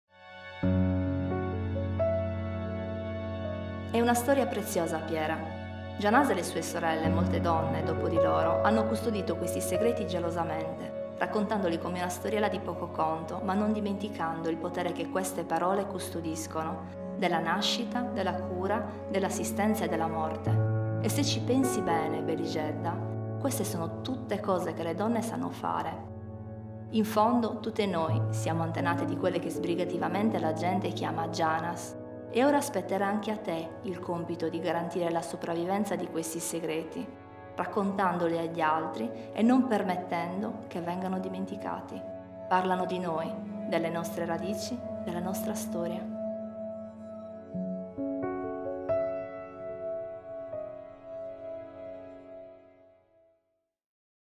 JANÀSA
– È una storia preziosa, Piera. Janàsa e le sue sorelle, e molte donne dopo di loro, hanno custodito questi segreti gelosamente, raccontandoli come una storiella di poco conto, ma non dimenticando il potere che queste parole custodiscono: della nascita, della cura, dell’assistenza e della morte. E se ci pensi bene, bellixedda, queste sono tutte cose che le donne sanno ancora fare. In fondo tutte noi siamo antenate di quelle che sbrigativamente la gente chiama jànas. E ora spetterà anche a te il compito di garantire la sopravvivenza di questi segreti, raccontandoli agli altri e non permettendo che vengano dimenticati. Parlano di noi, sono le nostre radici, la nostra storia.

Da Janàsa di Claudia Zedda
Edizioni Condaghes

Video realizzato da Here I Am
Per acquistare il libro